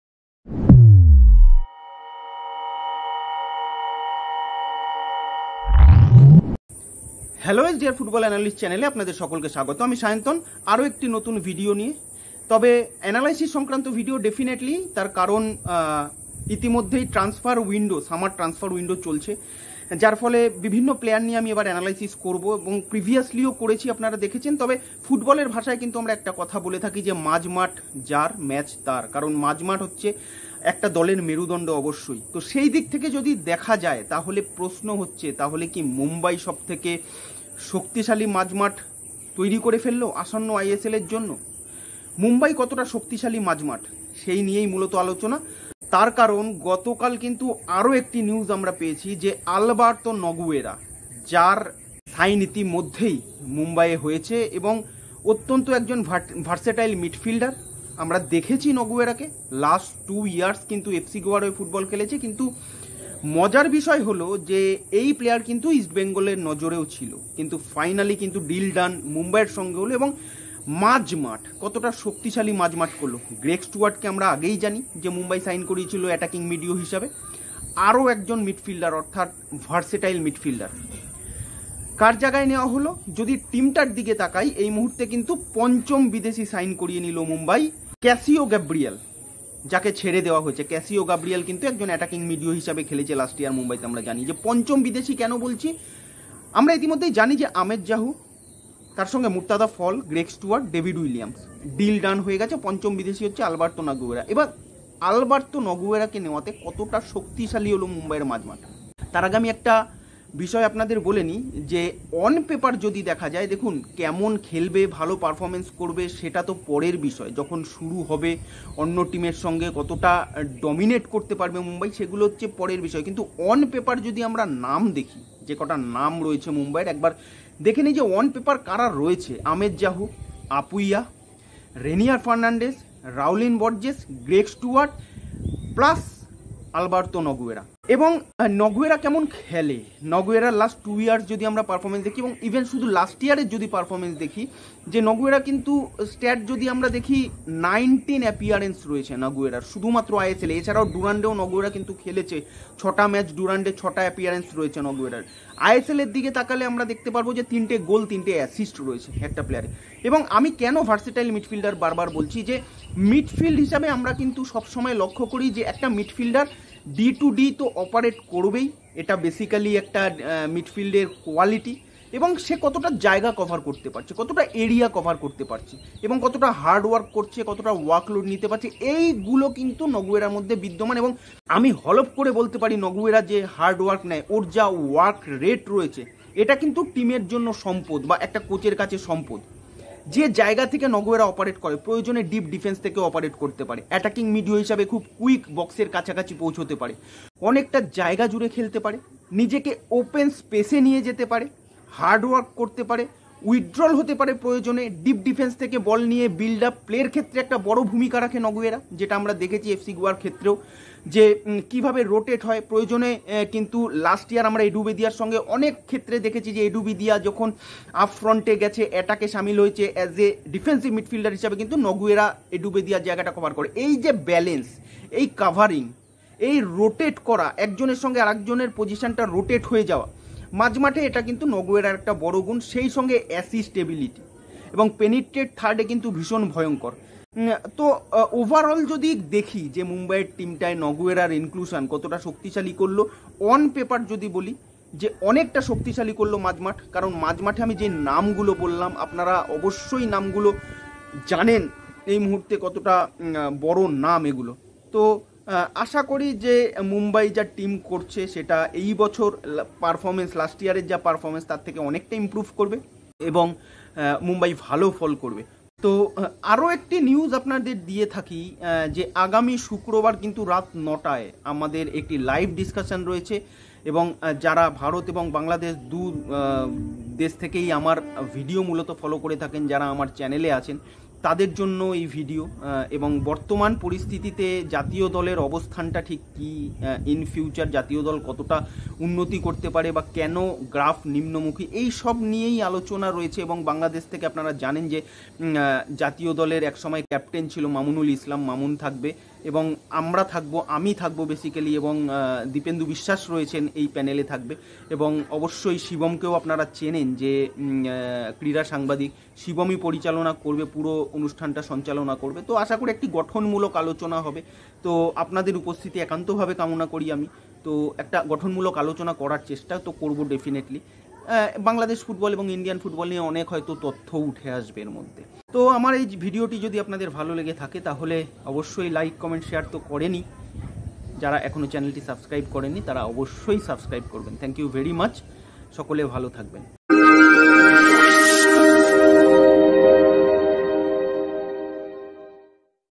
ট্রানস্ফার মার্কেটেও মাঝ মাঠ দখলের লড়াইয়ে এগিয়ে মুম্বাইয় // Alberto Naguera // Players Analysis
ইস্টবেঙ্গলের নজরে থাকা এফ সি গোয়ার ফুটবলার আলবার্তো নগুয়েরাকে সই করিয়ে নিল মুম্বাই। এখন থেকেই দলবদলে লড়াইয়ে মাঝমাঠে বাকি দলগুলো কে ডমিনেট করতে শুরু করেছে মুম্বাই। কেন নগুয়েরা কে দলে নিল মুম্বাই। বিশ্লেষণে সায়ন্তন দাস রায়।